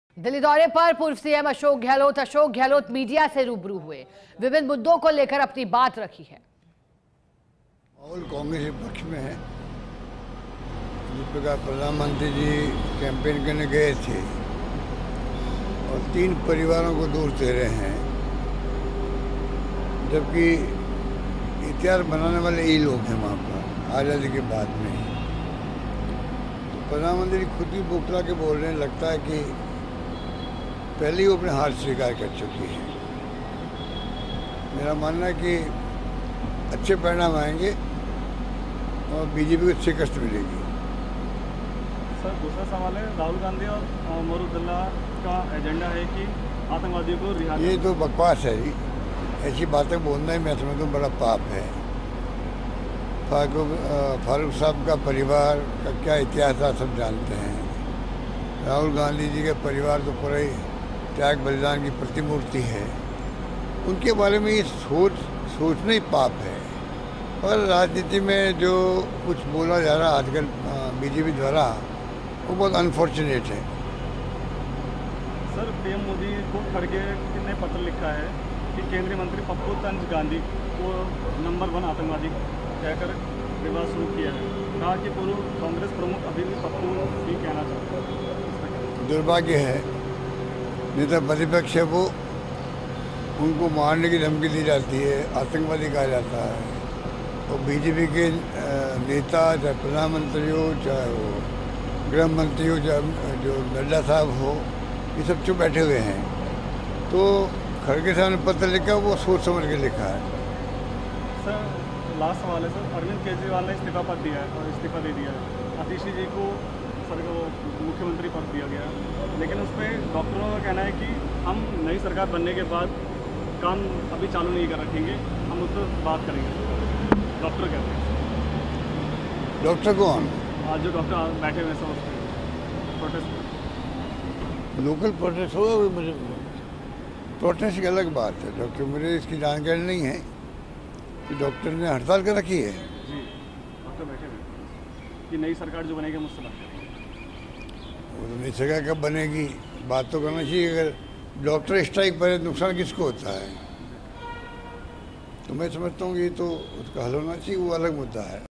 जम्मू-कश्मीर चुनाव को लेकर बोले Ashok Gehlot, कहा- माहौल कांग्रेस के पक्ष में | JK Election 2024
About this Video:
 जम्मू-कश्मीर चुनाव पर कांग्रेस नेता और राजस्थान के पूर्व मुख्यमंत्री अशोक गहलोत ने कहा, "माहौल कांग्रेस के पक्ष में है। प्रधानमंत्री मोदी तीन परिवारों को दोष दे रहे हैं जबकि वहां पर इतिहास बनाने वाले यही लोग हैं। प्रधानमंत्री खुद बौखला कर बोल रहे हैं। ऐसा लगता है कि वे पहले ही अपनी हार स्वीकार कर चुके हैं, अच्छे परिणाम आएंगे, भाजपा को शिकस्त मिलेगी...राजनीति में आजकर भाजपा द्वारा जो कुछ बोला जा रहा है वो बहुत दुर्भाग्यपूर्ण है | 18 September 2024